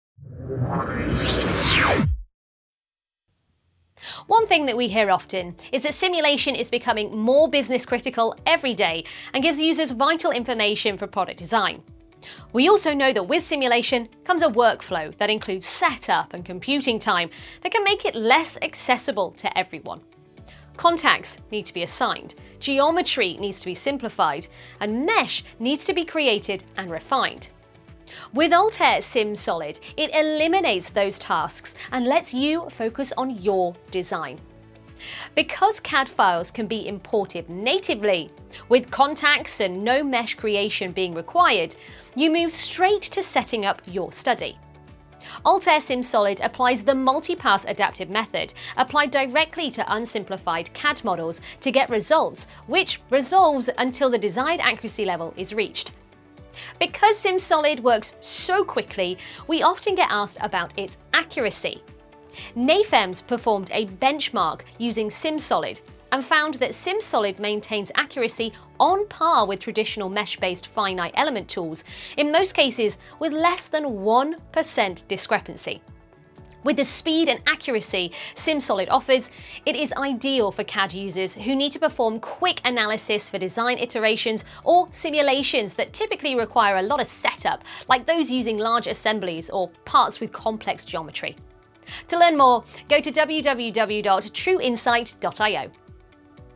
What is Altair SimSolid?
Altair SimSolid allows users to bypass typical time consuming steps of geometry simplification and mesh creation and go straight to setting up the study.  This is ideal for those who need quick design iterations or those working on large or complex assemblies.